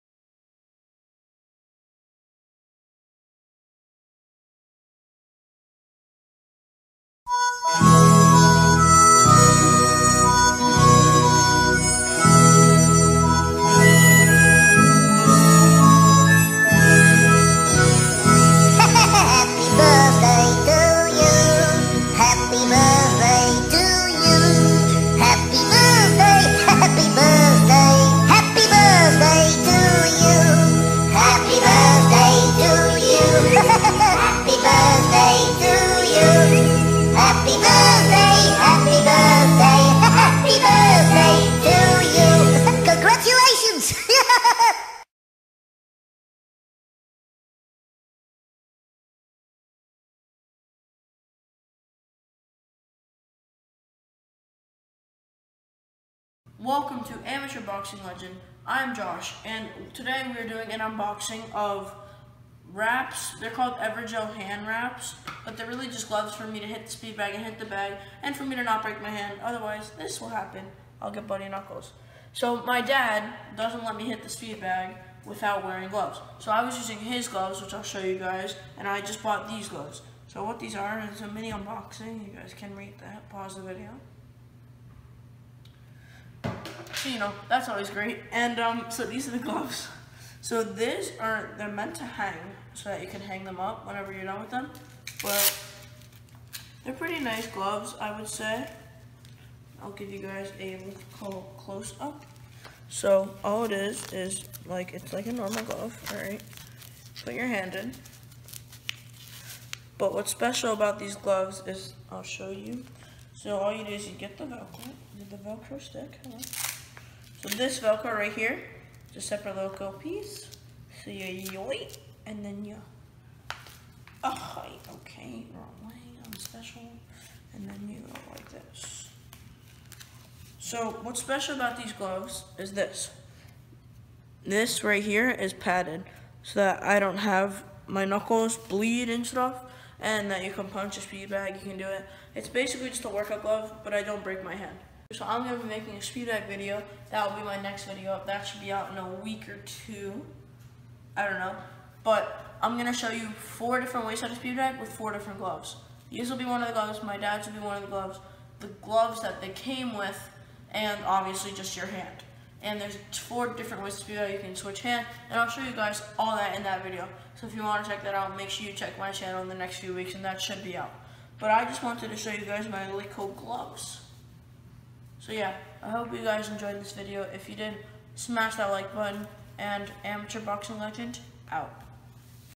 Unboxing Speedbag Gloves
In this video I will be showing you how to unbox speedbag gloves